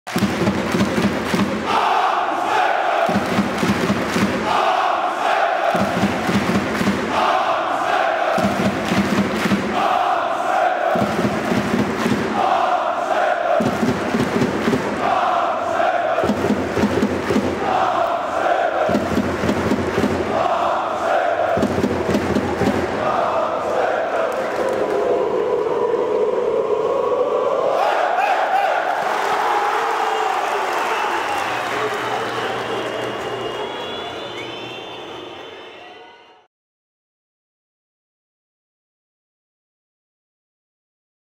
SPILLERSANG: MARVIN SCHWÄBE
Der var et nervepirrende topopgør på menuen, da topholdet fra FC Midtjylland gæstede Brøndby Stadion og med en sejr kunne spille sig et stort skridt nærmere det danske mesterskab. Her kan du se Marvin Schwäbes fenerbahce fra kampen.